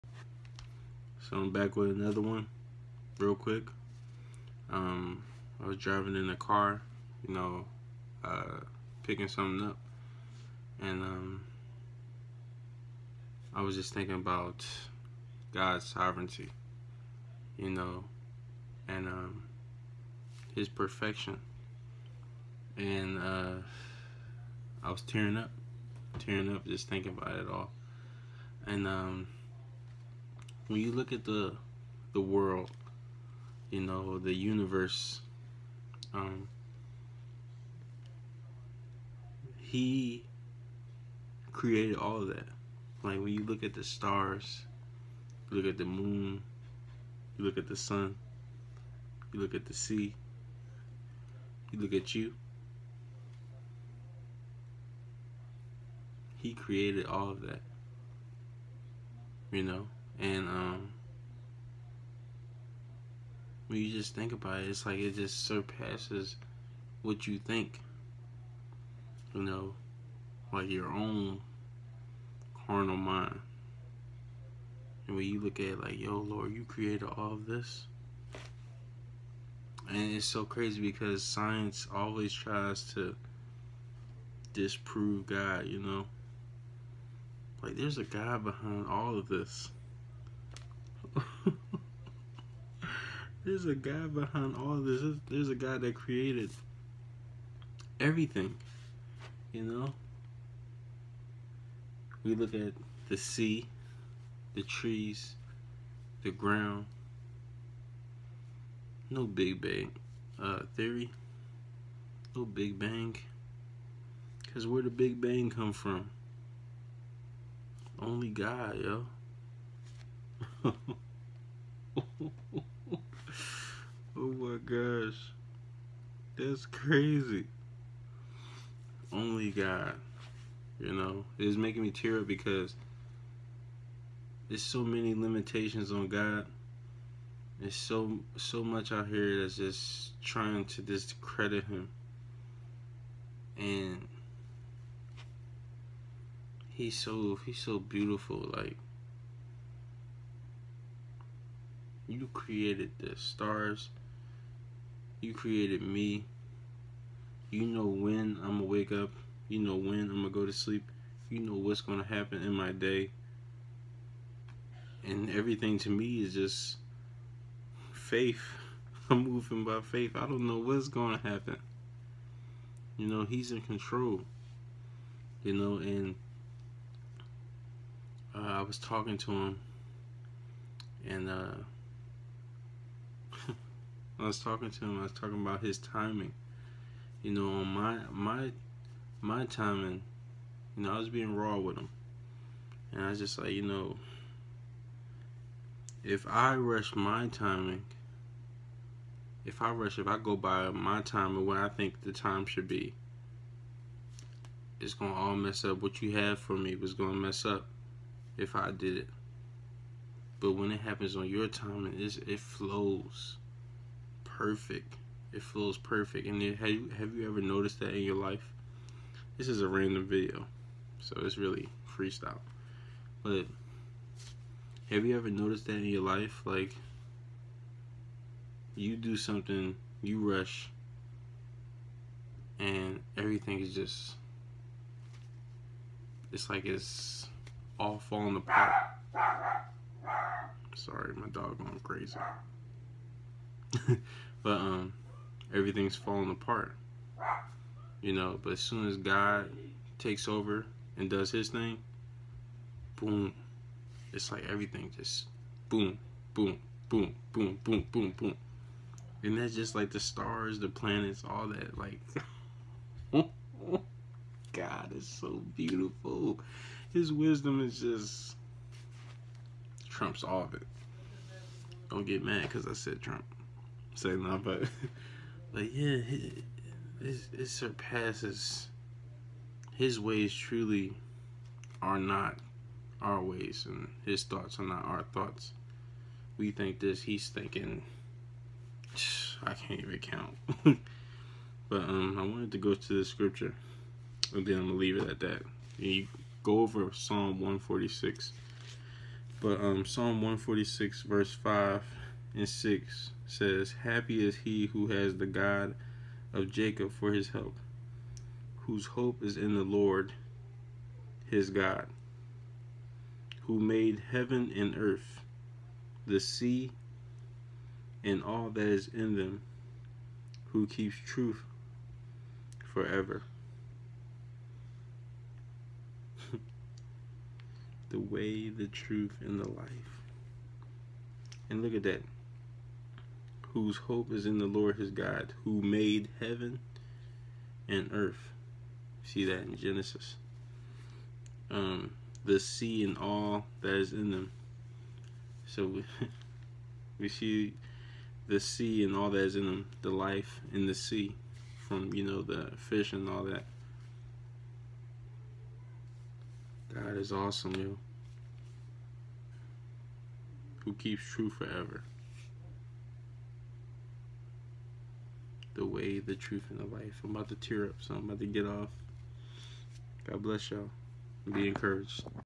Beautiful God🙌🏾
Praise the Lord for He is wonderful and Almighty and perfect! Hallelujah 🙌🏾

Psalm 146:5-6- Happy is he who has the God of Jacob for his help, Whose hope is in the Lord his God,
6. Who made heaven and earth, The sea, and all that is in them; Who keeps truth forever,